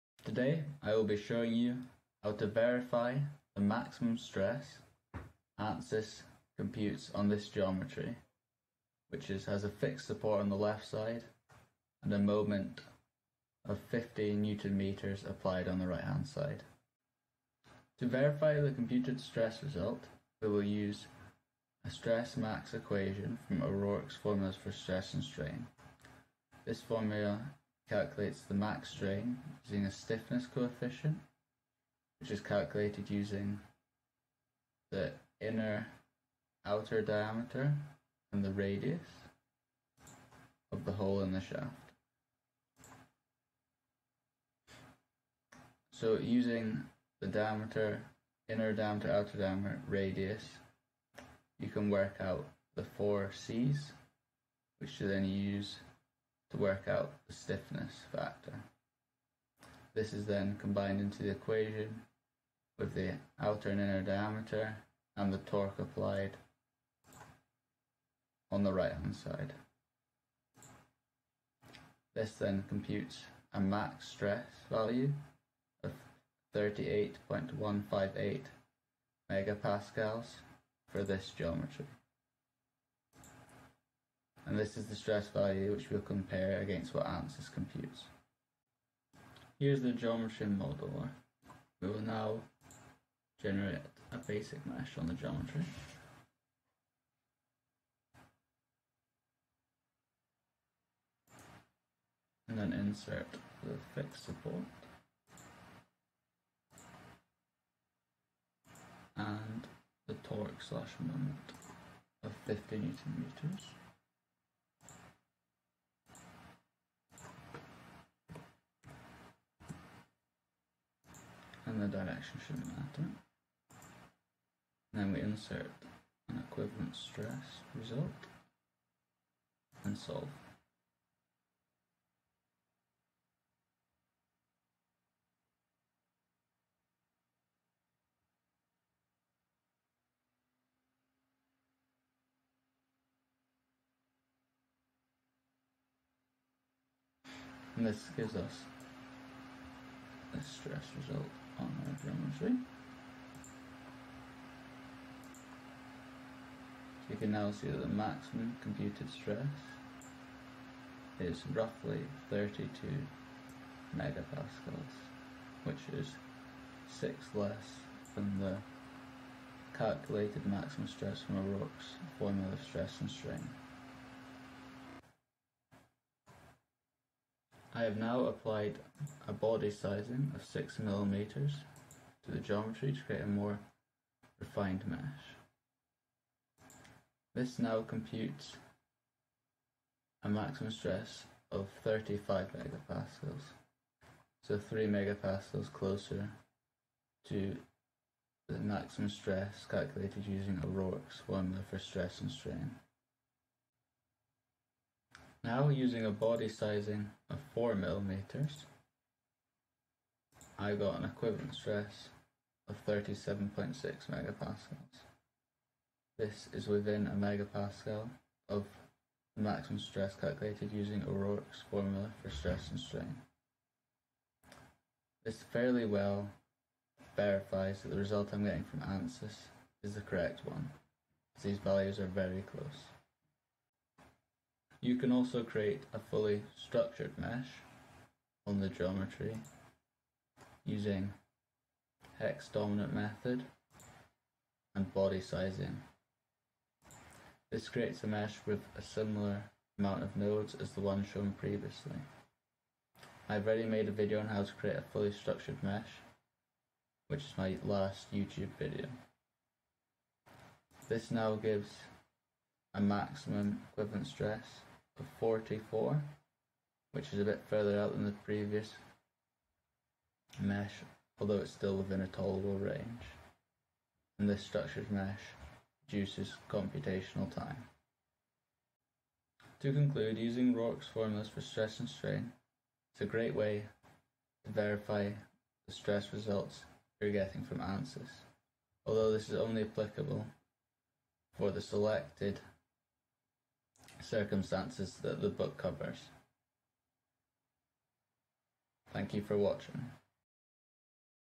ANSYS vs Hand Calculations | FEA Results Verification Tutorial
This video shows how to create a structured hexahedral mesh in Ansys Mechanical. The method used is efficient and effective for any geometry possible.